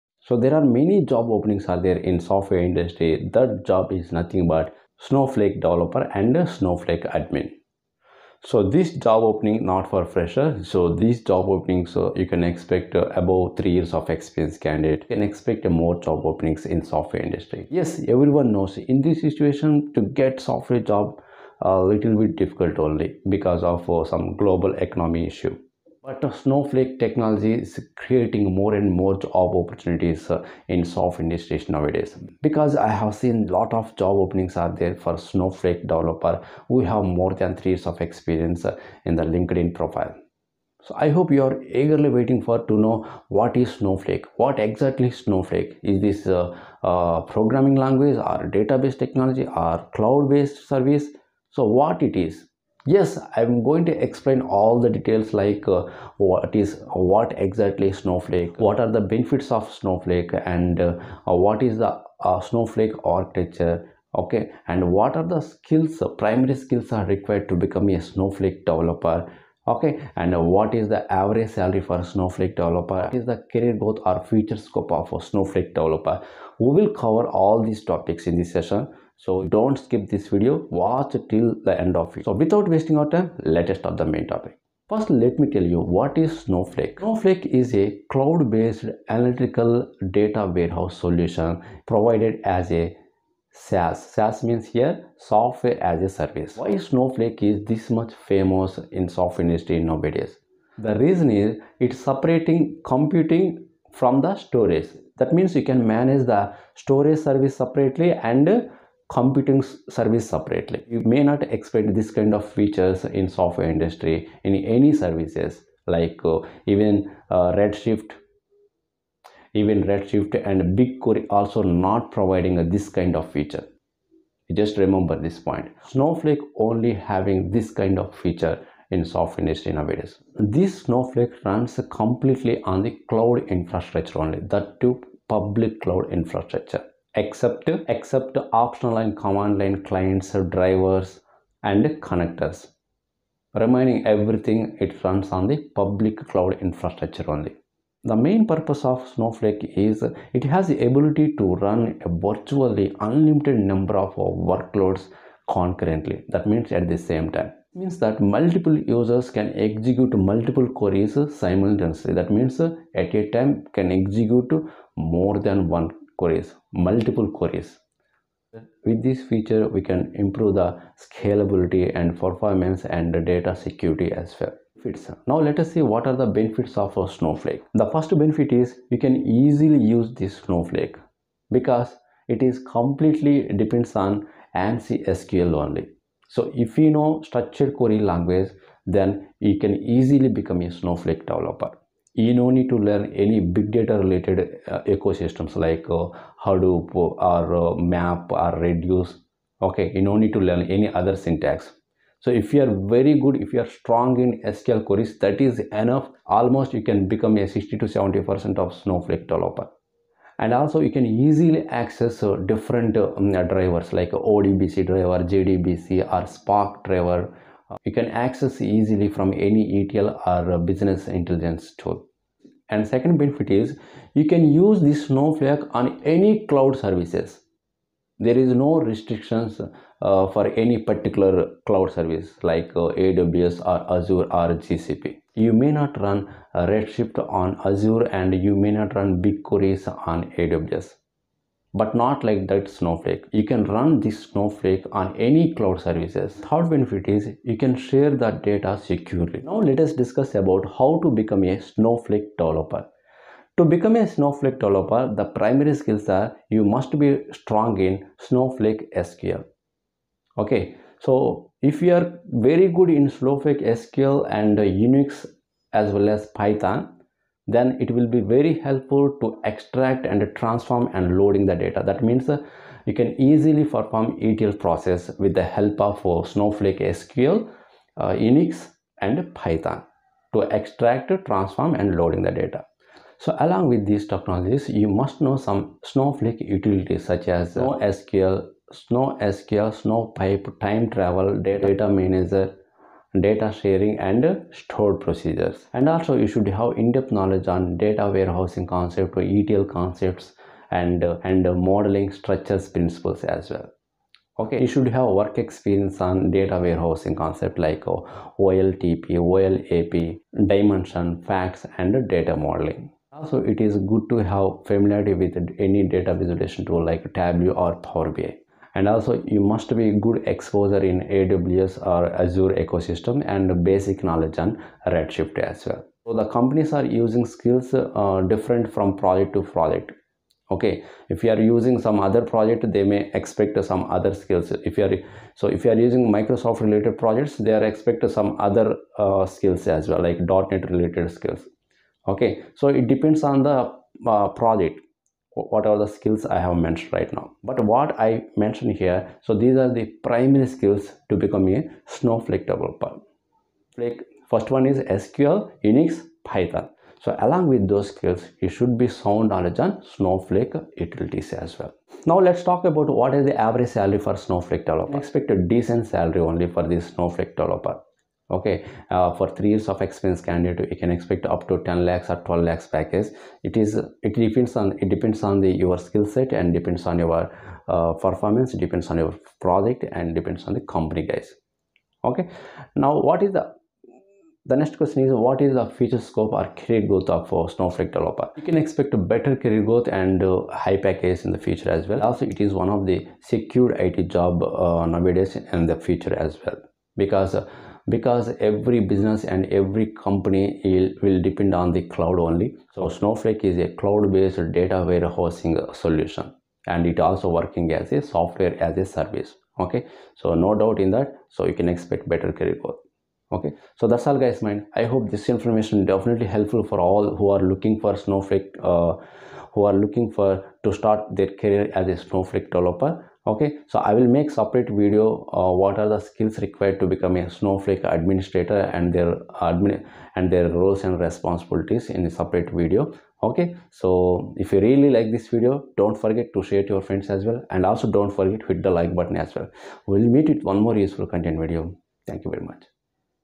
How To Become Snowflake Developer | Scope of Snowflake | Snowflake Tutorial
What is Snowflake, benefits of Snowflake, Snowflake architecture, what are the skills required to become Snowflake Developer, Snowflake Data engineer, career growth of Snowflake and average salary of Snowflake Developer.